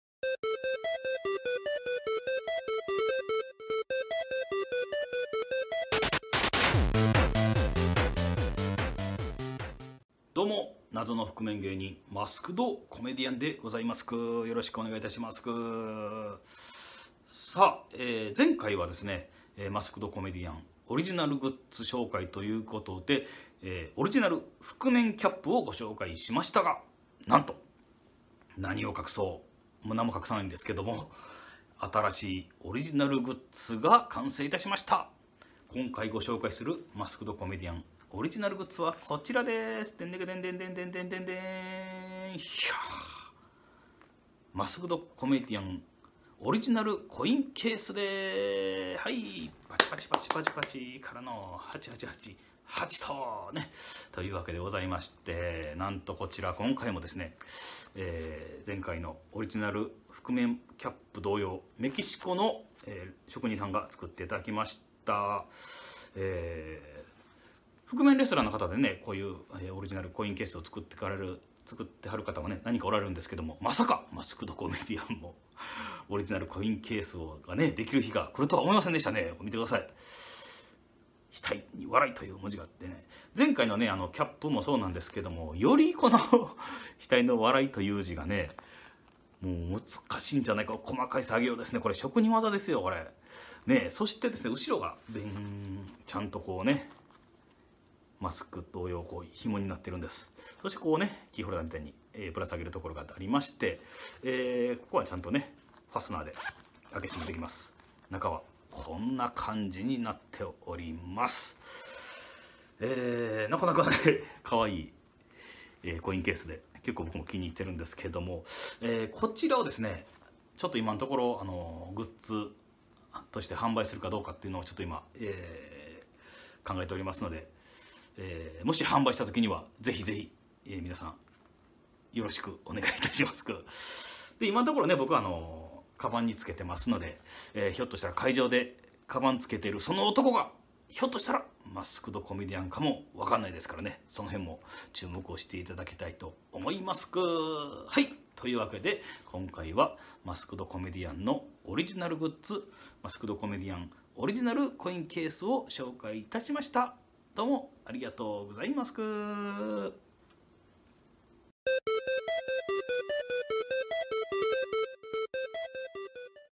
覆面ワールド劇場#10～マスクド・コメディアンのオリジナル覆面コインケース紹介～
覆面ワールド劇場#10～マスクド・コメディアンのオリジナル覆面コインケース紹介～(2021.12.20)

謎の覆面芸人、マスクド・コメディアンpresents覆面ワールド劇場」です。

今回は、「マスクド・コメディアンのオリジナル覆面コインケース紹介」です。

『マスクド・コメディアンのSNS』